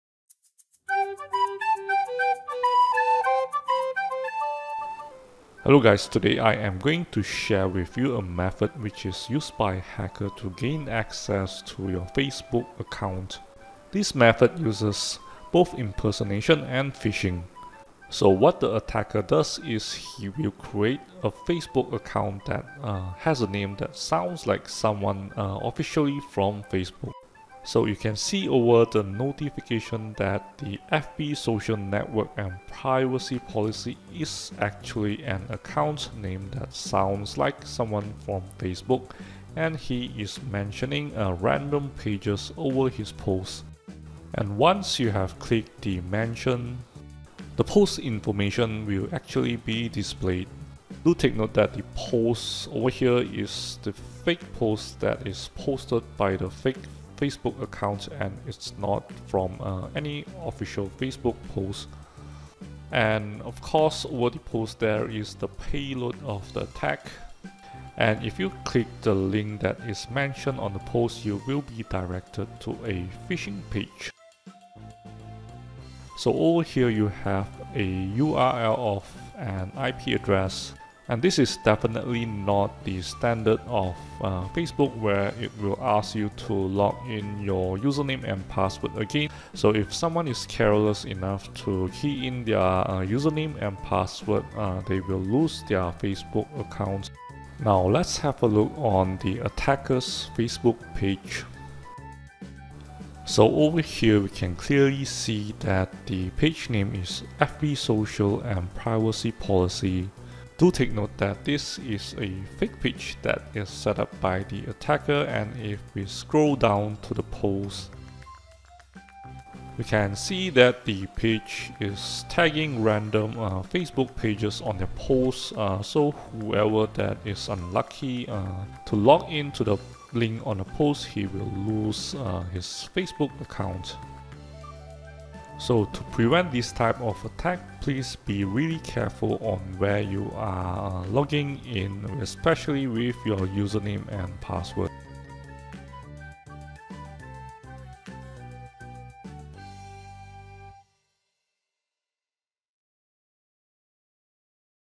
Facebook Phishing posts attack explained
Facebook Phishing posts attack explained. How an attacker/ Hacker uses Facebook mention to trick people into signing into fake Facebook login pages.

00:08 What is Facebook Mention
00:23 Face Facebook page  will mention mass pages
00:55 Fake post with Phising link
01:22 Phising site